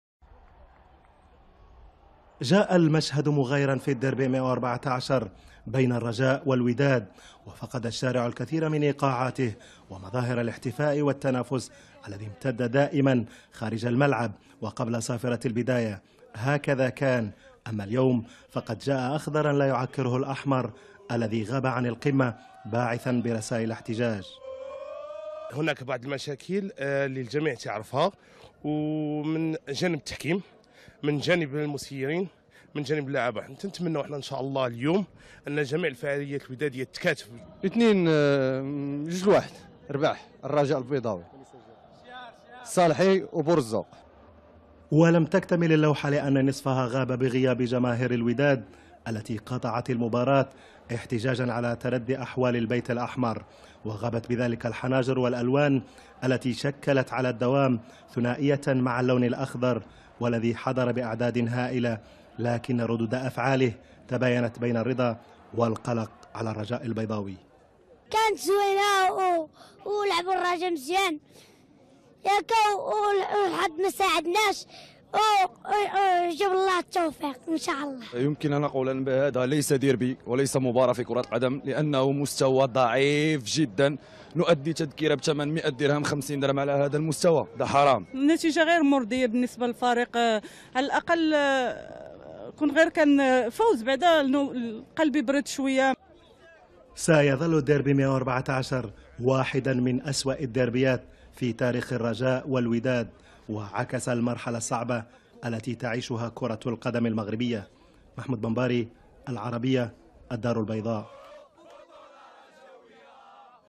Le prasson- Derby114 الديربي البيضاوي 114 على العربية واستجواب مع لوبغاسو
. ملخص من قناة العربية واستجواب مع لوبغاسو الرجاوي الحقيقي !!